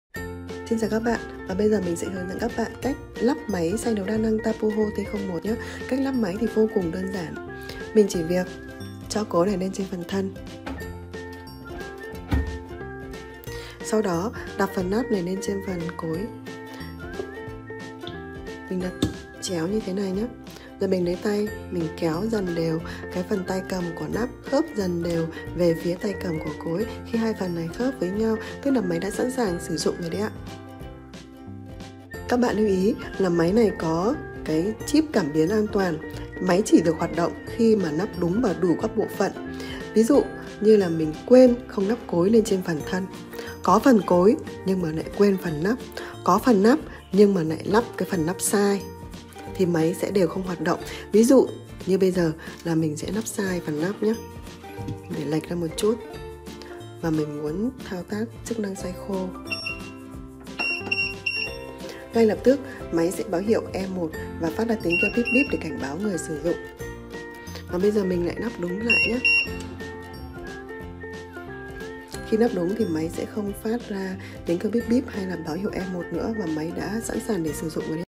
Hướng dẫn lắp máy xay nấu đa năng TAPUHO T01
🌺Cách lắp máy vô cùng đơn giản
- Cho cối lên trên phần thân
- Sau đó lắp phần nắp lên phần cối chéo và kéo về phía tay trái cho vừa nắp máy
💥Lưu ý: phải lắp chặt và đúng cách không máy sẽ không hoạt động
Máy xay nấu đa năng TAPUHO T01
💥 Bảo hành chính hãng 24 tháng.
💥 Đổi mới  trong 10 ngày đầu.
💥 Freeship Toàn Quốc.